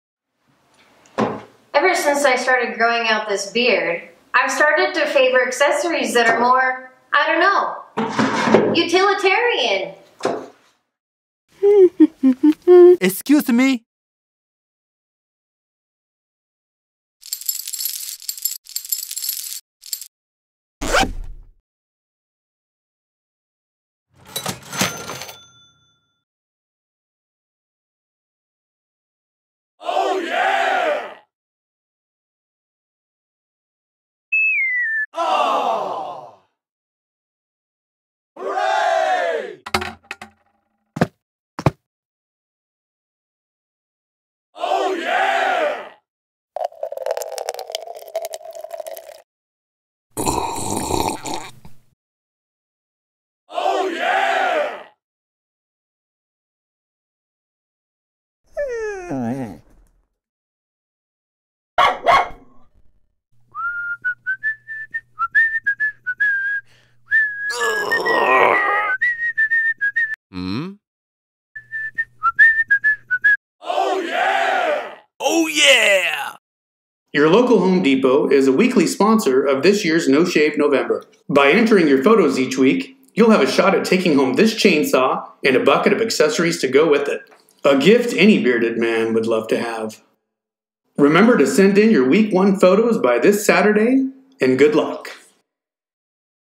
No-Shave November 2017 - Home Depot (Week 1)
Ever since Lillian decided to grow out a beard, she has begun to favor accessories that are more. utilitarian, you might say.